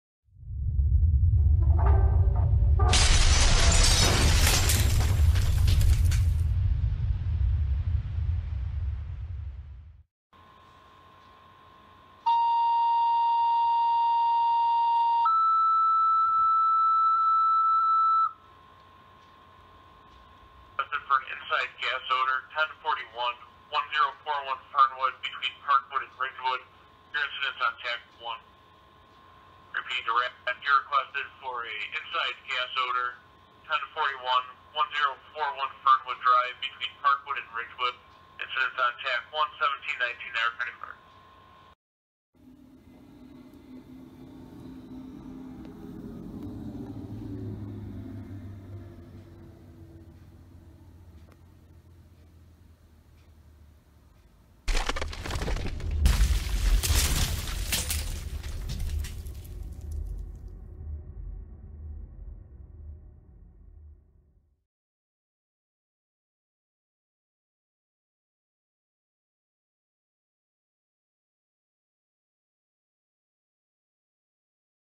Rapids VFC 19-E1 Responding Cold to a Gas Odor Investigation | 7/18/18
*Recorded 7/12/18 @ 17:32
Rapids Volunteer Fire Company 19-E1 is shown responding cold to a gas odor investigation in Lockport, NY.  19-M7 also responded, but is not shown in this video. Dispatch audio is included.

Timeline:
-17:34 19M7, 19E1 on location
-17:41 Checked for readings, nothing found, Rapids clear/returning

Rapids VFC is a 100% volunteer fire company located in Lockport, NY.  They have an annual call volume of approximately 1000+ calls/year.

19-E1 is a 2001 Pierce Enforcer Rescue Pumper (1000g/1750 gpm)